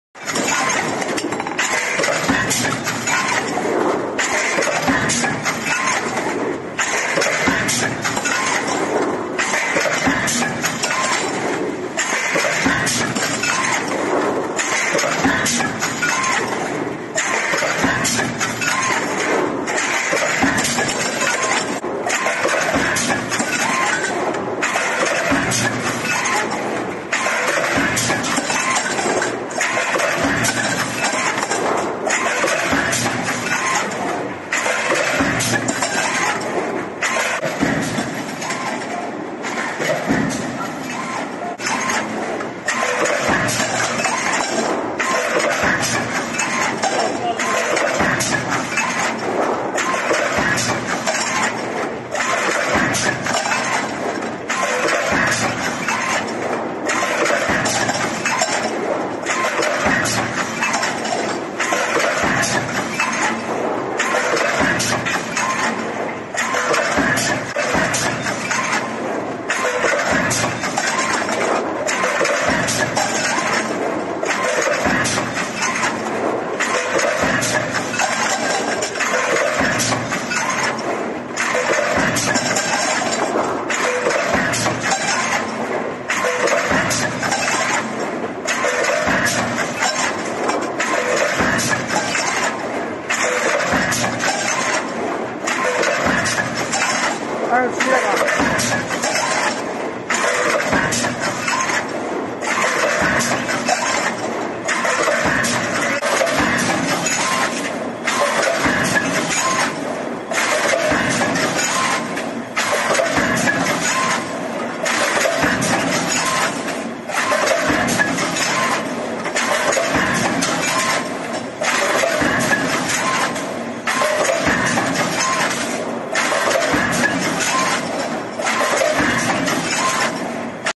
Automobile filter production line is stamping, machine with manipulator
Besco Machine Tool Limited